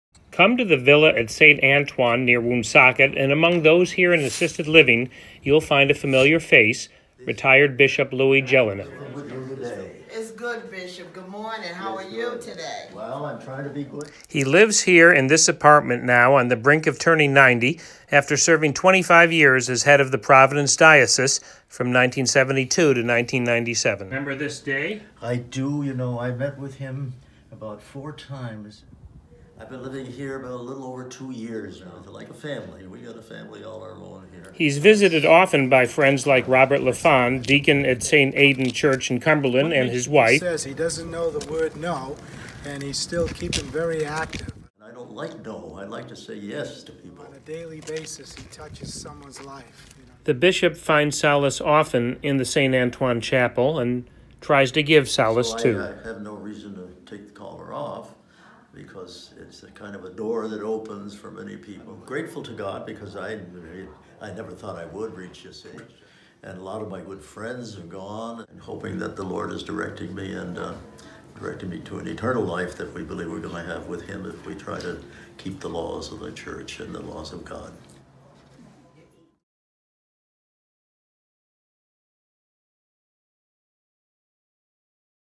Visiting with Bishop Gelineau
The Villa at Saint Antoine, North Smithfield, RI Providence Journal video by Mark Patinkin